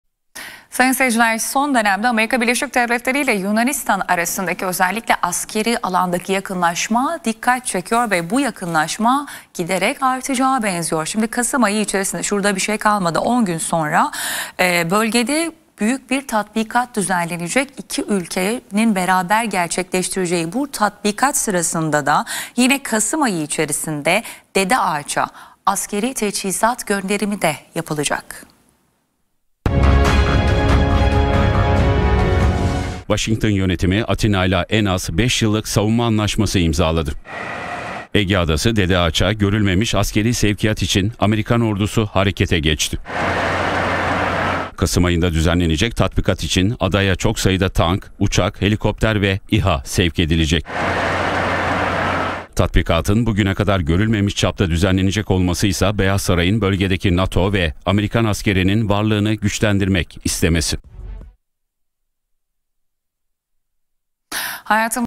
ABD - Yunanistan ilişkisi büyüyor! Dedeağaç'a dev sevkiyat başlayacak
TÜRKSAT 4A
Frekans: 12729
Sembol: 30000
Polarizasyon: V (Dikey)